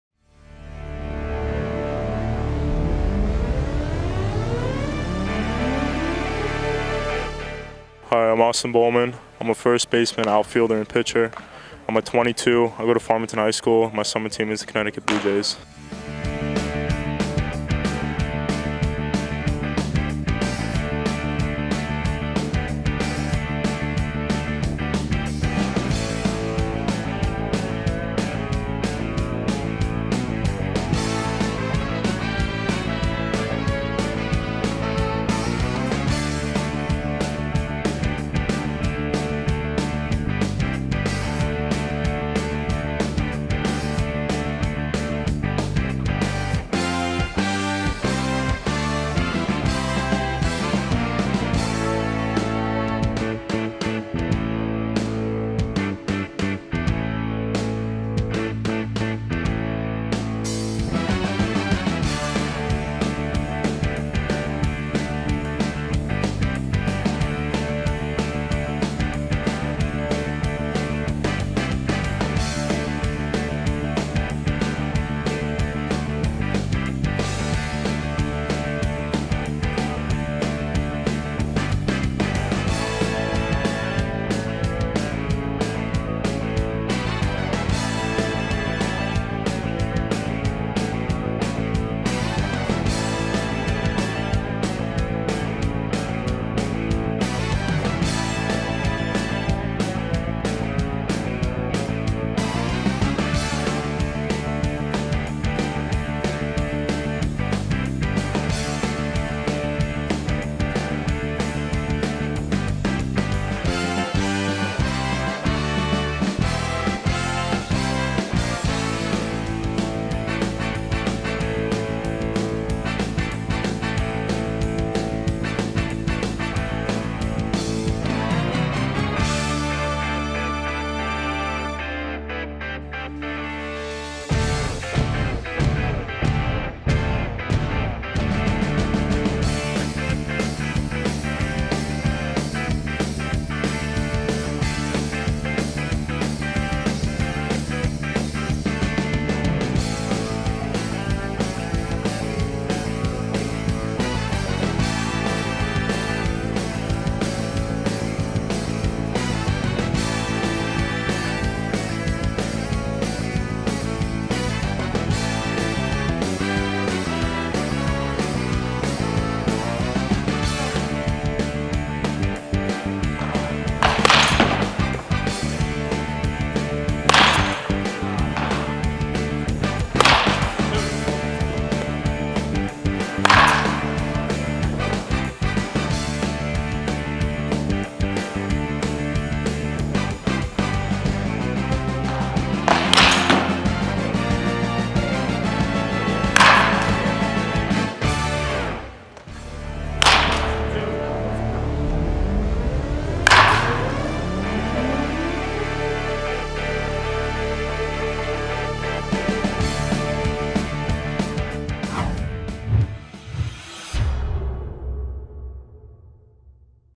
Austin Bohlman - 1B/OF/LHP - Farmington, CT - 2022
Perfect Game New England Indoor Showcase filmed by Skillshow. The Shrewsbury Club. Shrewsbury, MA. March 6 - 7, 2021.